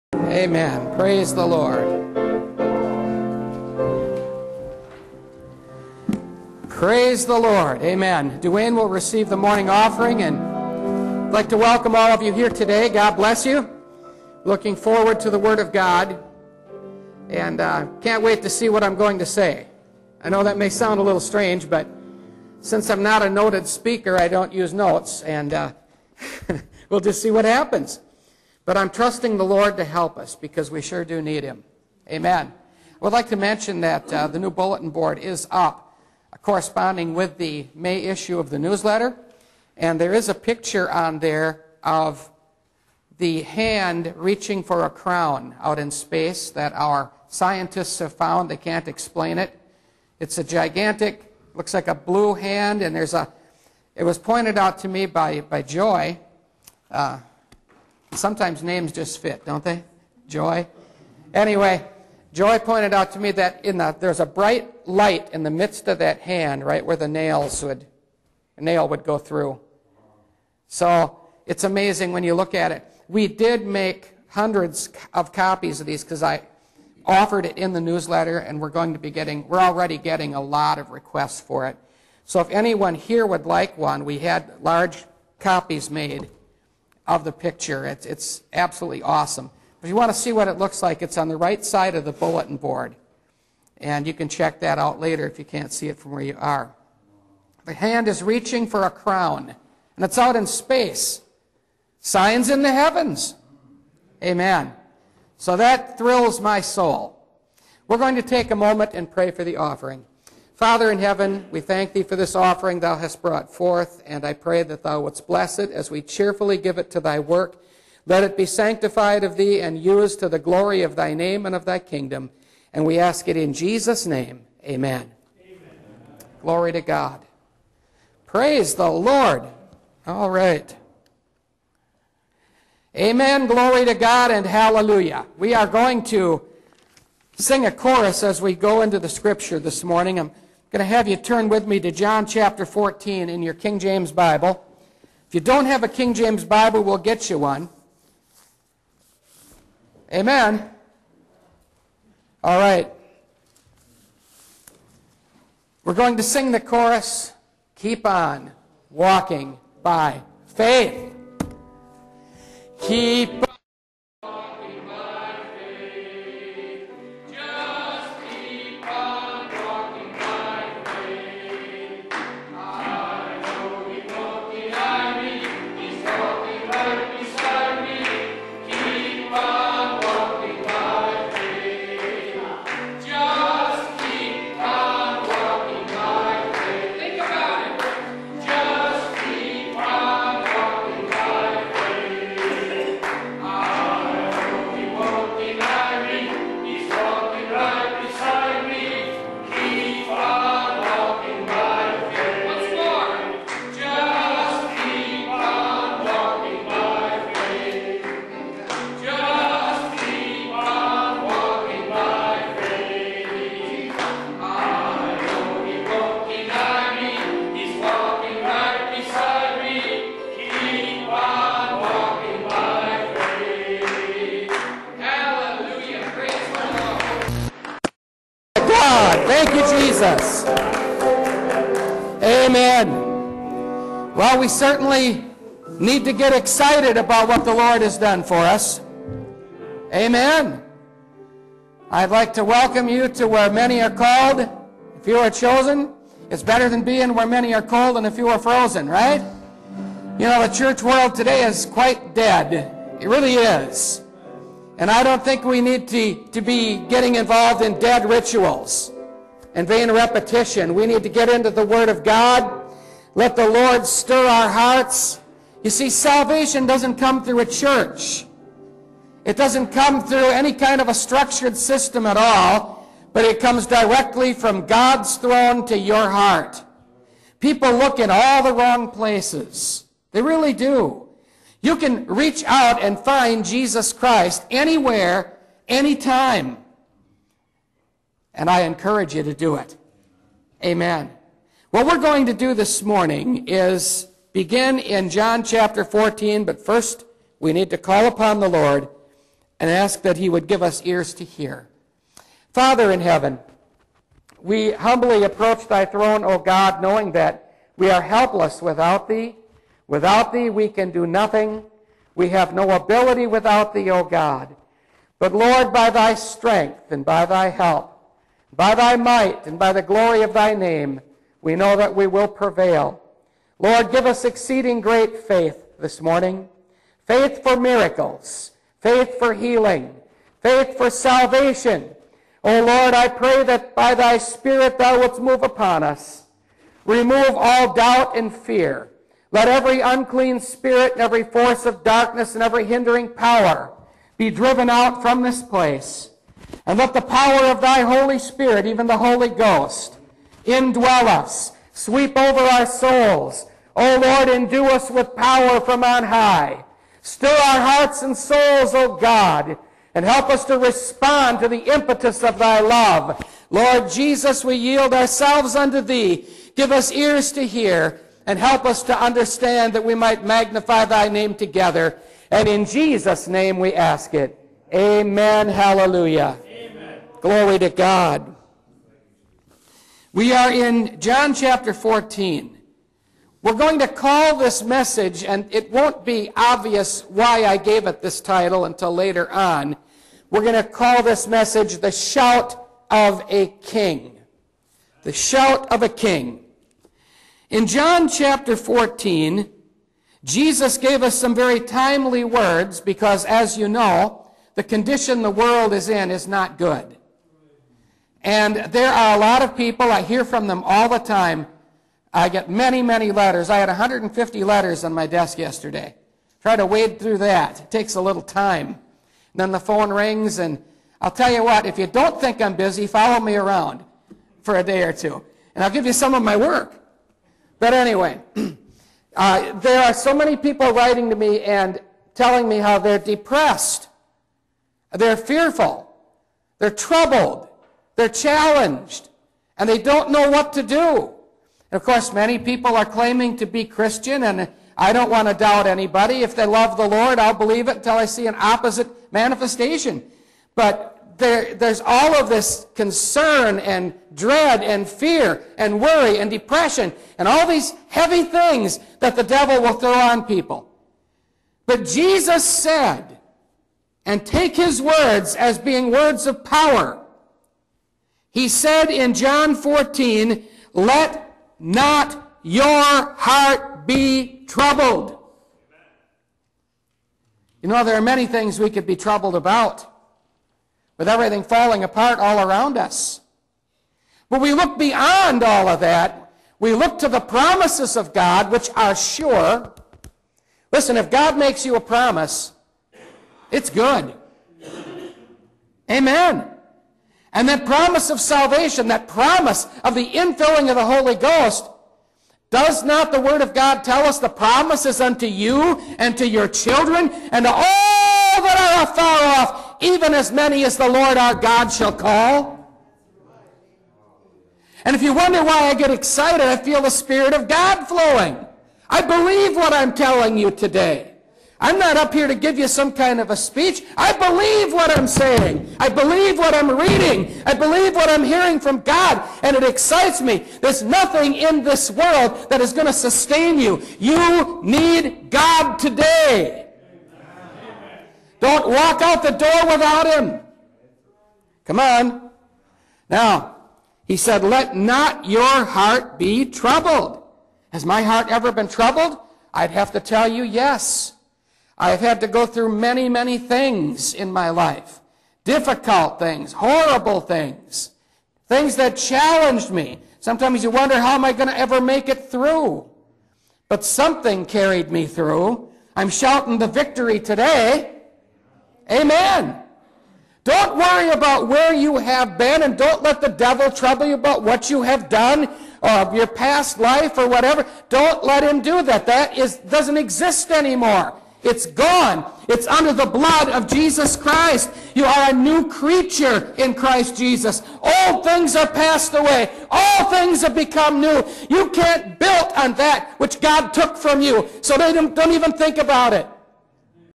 Last Trumpet Ministries:The Shout Of A King Part 1
Is it not amazing that we have a king of kinks and lord of Lords. Those whom God has called forth to repentance were miserable sinners. Why did God choose not to destroy us for our disobedience? Because the shout of a king, Jesus Christ is in us.